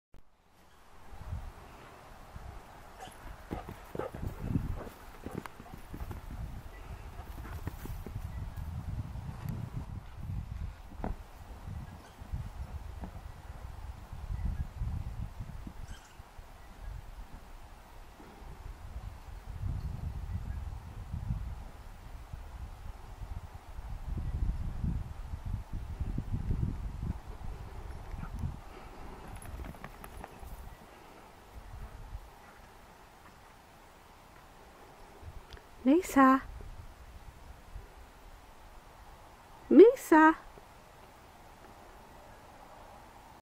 Maysa Zooming & Digging
Maysa doing zoom zooms & digging in her kennel